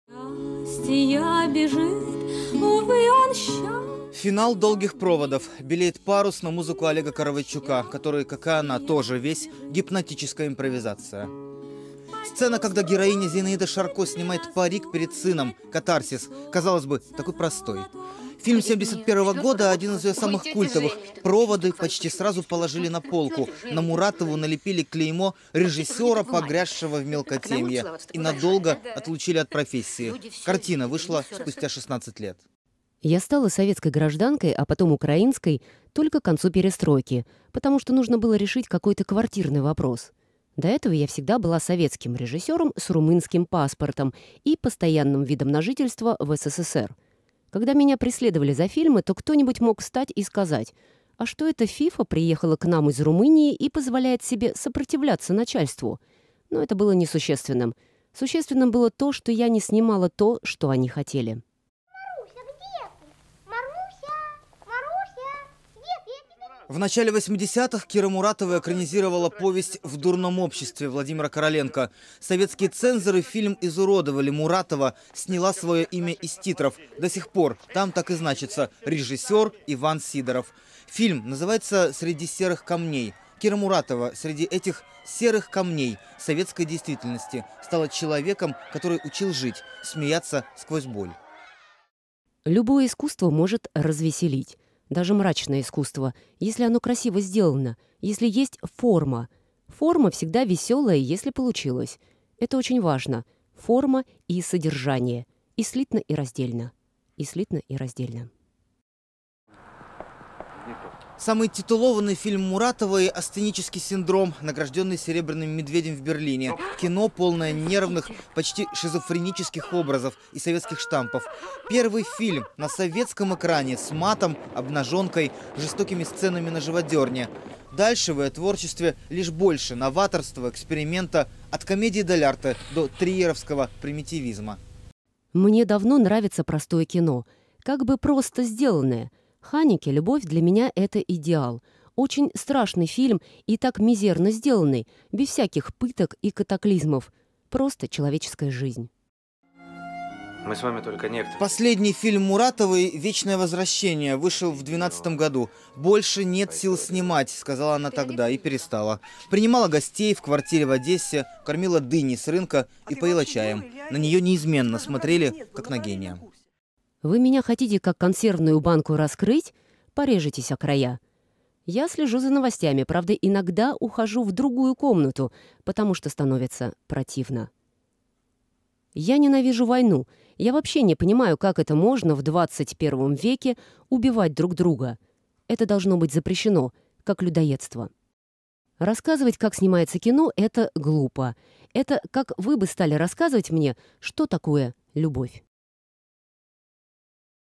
Кира Муратова. Памяти гения — ее фильмы и ее слова
Более чем за 50 лет карьеры Кира Муратова сняла 20 фильмов, а в 80 лет режиссер завершила карьеру